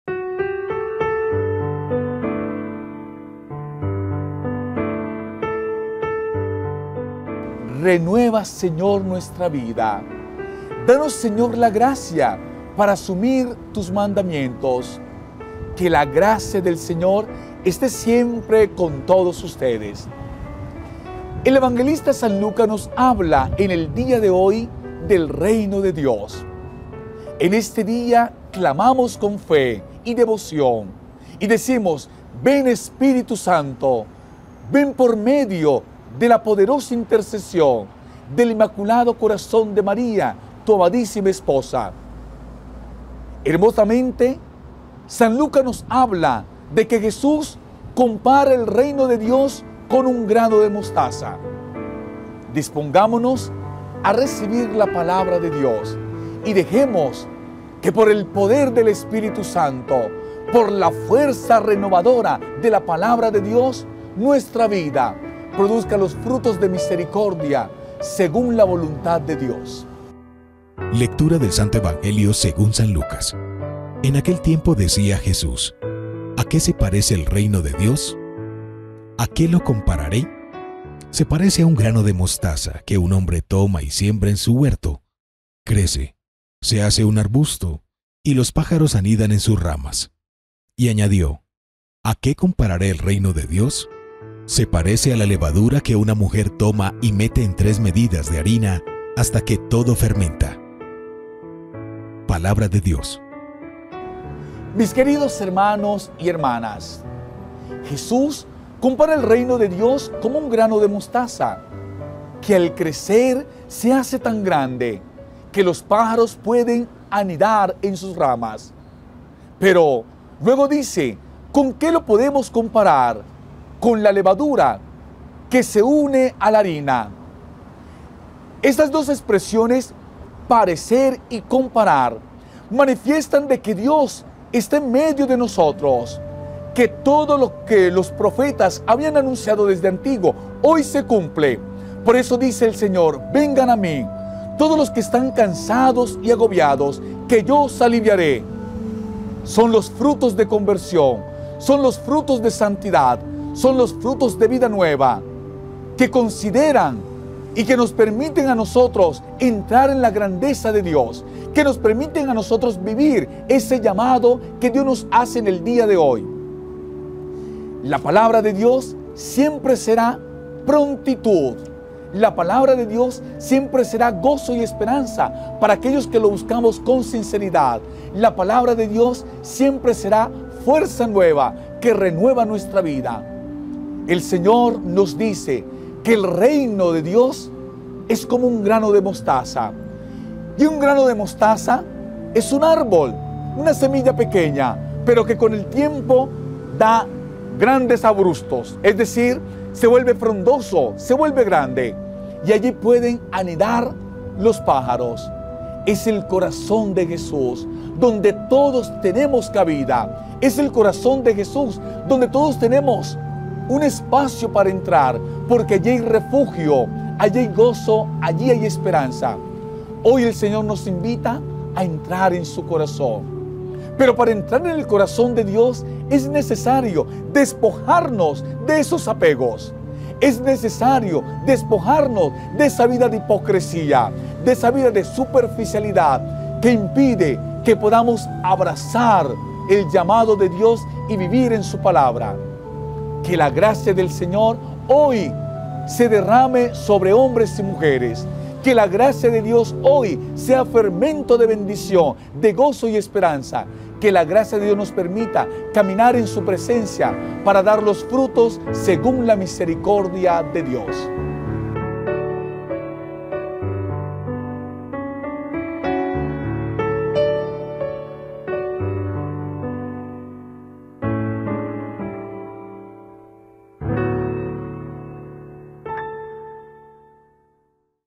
EVANGELIO DE HOY, Martes 25 De Octubre De 2022 - Cosmovision
En aquel tiempo, , decía Jesús:
«¿A qué es semejante el reino de Dios o a qué lo compararé?
Es semejante a un grano de mostaza que un hombre toma y siembra en su huerto; creció, se hizo un árbol y los pájaros del cielo anidaron en sus ramas».
Y dijo de nuevo:
«¿A qué compararé el reino de Dios?
Es semejante a la levadura que una mujer tomó y metió en tres medidas de harina, hasta que todo fermentó».